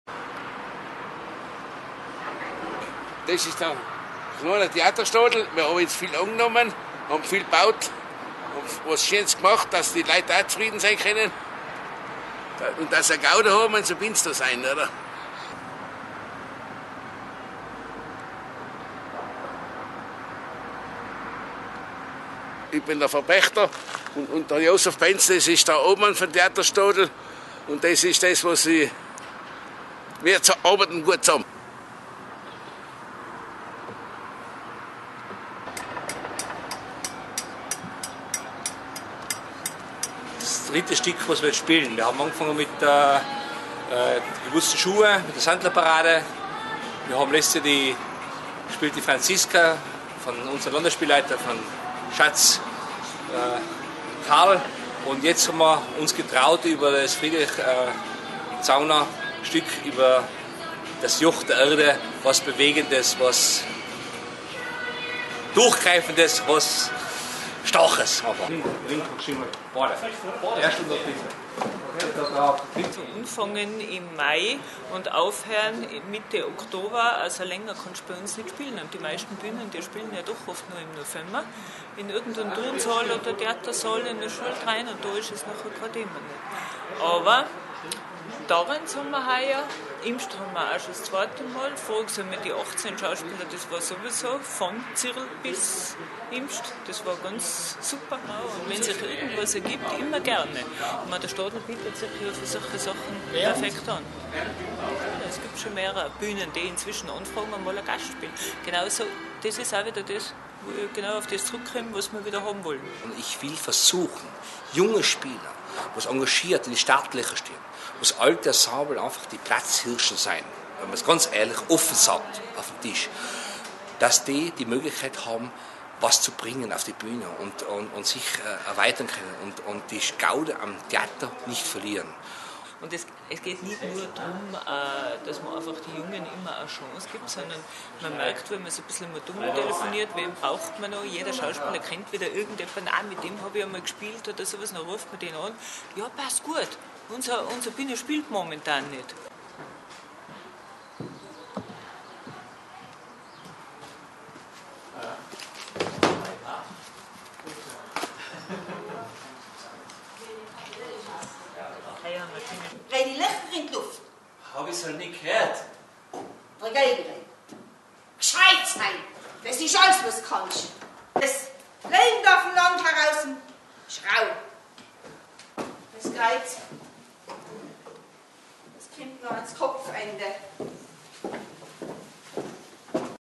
Das Kleine Bezirkstheater
Das Stadltheater in Stams und sein Programm. Clip: e.s.